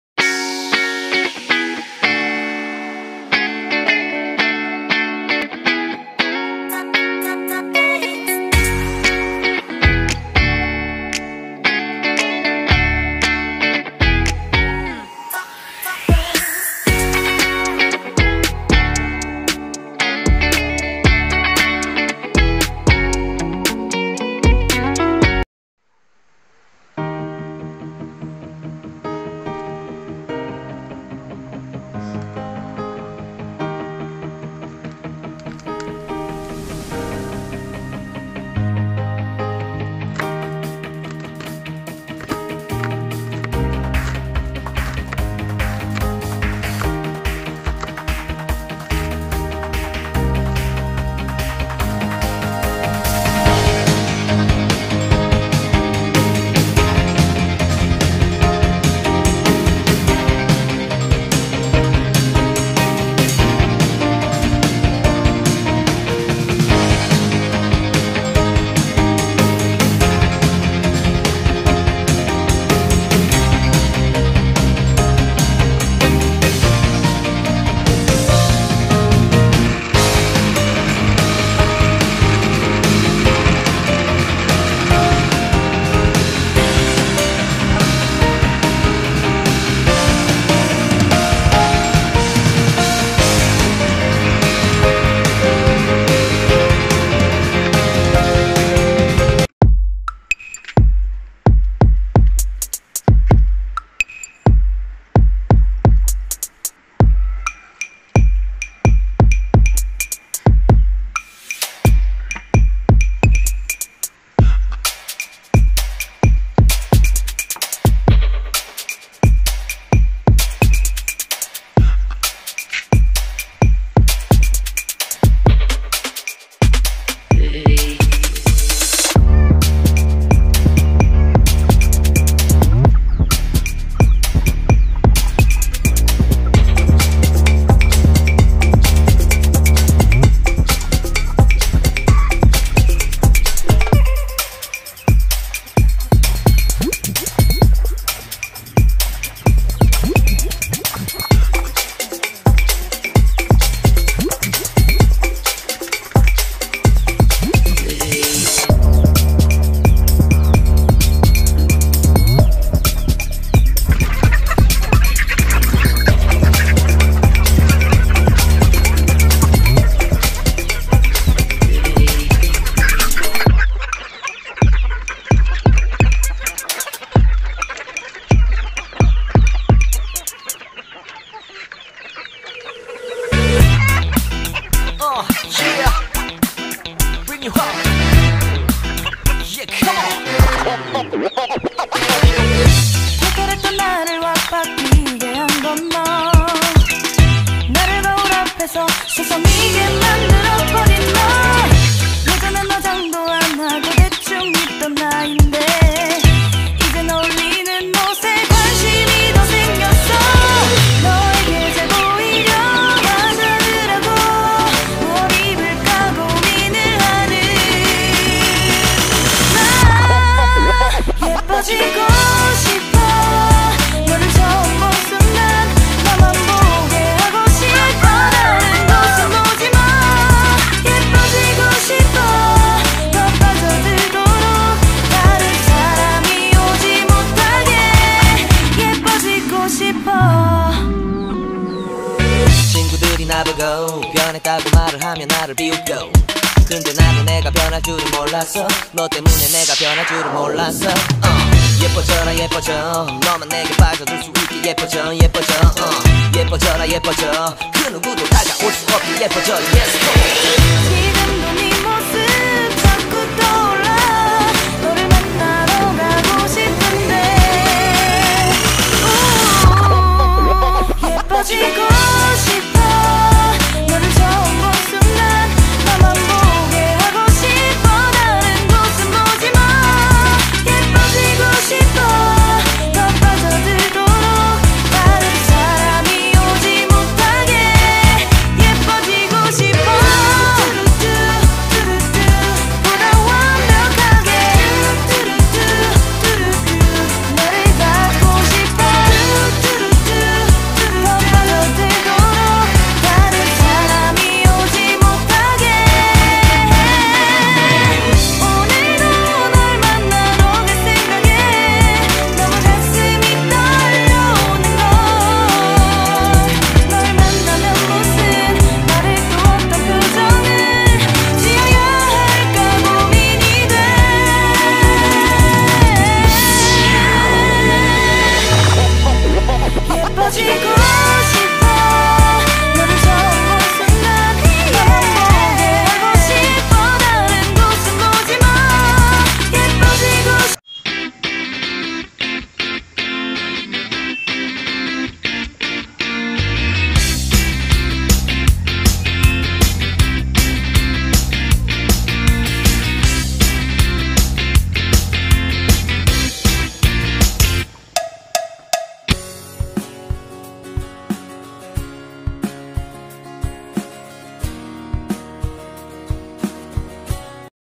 AWAS ❗❗❗ Setelah Coba Ayamnya jadi Ketagihan ✅ || Update Pakan Alternatif AYAM KAMPUNG
Pakan Alternatif untuk Ayam kampung. Disini saya masih favoritkan Singkong karena bahannya mudah didapatkan.